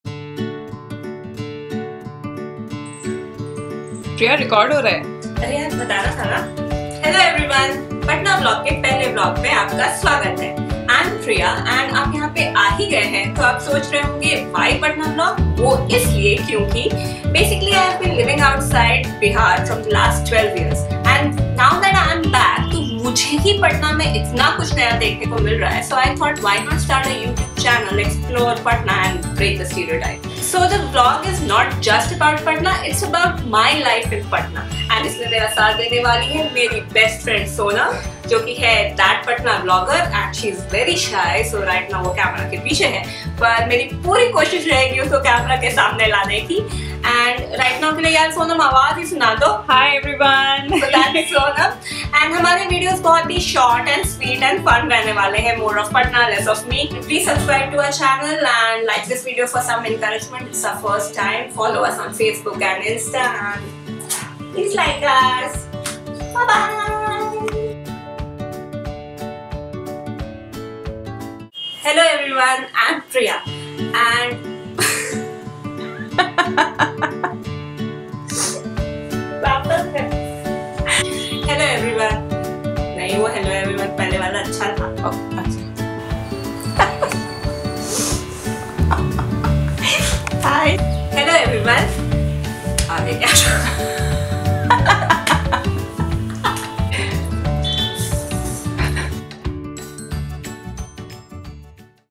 Patna Shots | Introduction | Lifestyle Vlogging in Patna
Welcome to Patna's first lifestyle YouTube channel. As 'BBFs turned vloggers' we will be posting exciting vlogs on anything and everything about Patna.

Much Love,
Pria and Sonam